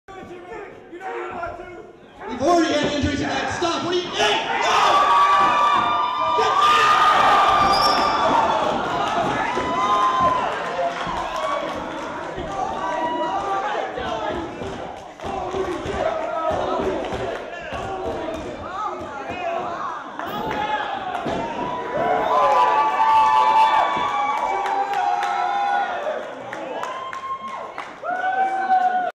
Jimmy Karryt does crowd control
After the UWC:104 main event and the brawl that ensued, Jimmy Karryt hit his patented "Gold Star Press" onto Phabian Barbosa and the group holding him back.